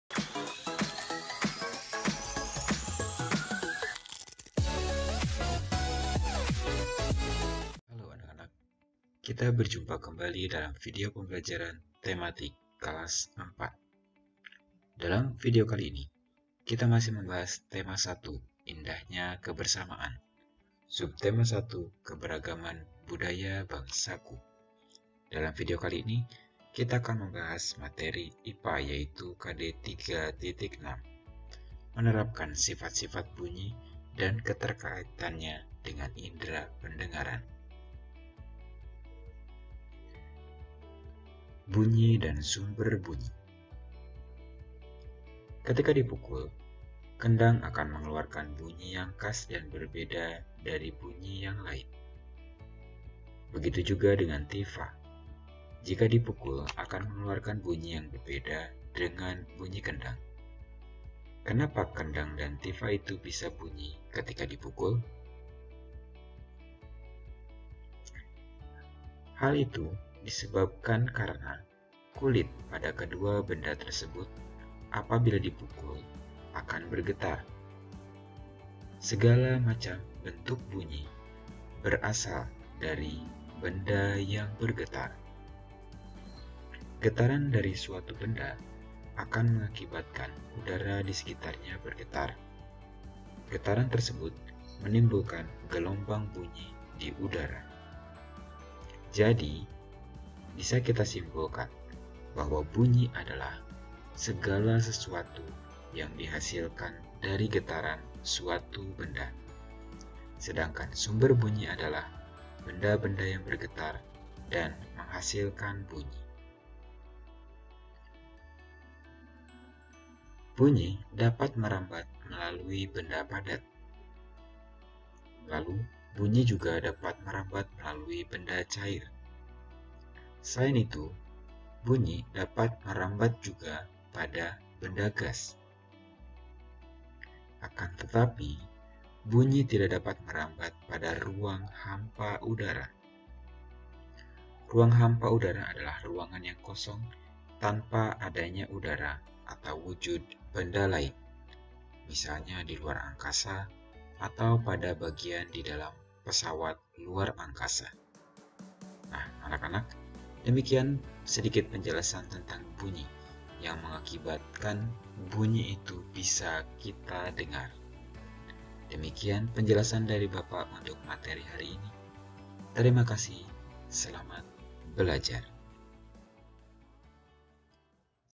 IPA Kelas 4_Tema 1 Subtema 1
Berikut ini adalah video materi IPA tentang bunyi dan sumber bunyi.